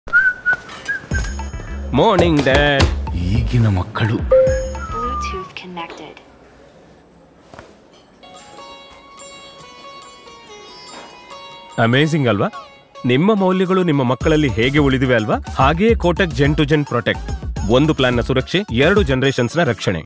Kotak Gen2Gen Protect - 20 sec - Kannada
Kotak Gen2Gen Protect is an industry-first protection plan that covers two generations* under one plan.

Now pass on the legacy of protection with Kotak Gen2Gen Protect; One plan’s security that ensures two generations’ safety*!

*T&C applied. The plan comes with two plan options: Legacy ROP and Life ROP. On opting for the Legacy ROP option, this plan provides life cover to the parent and after that to the identified child up to the specified age.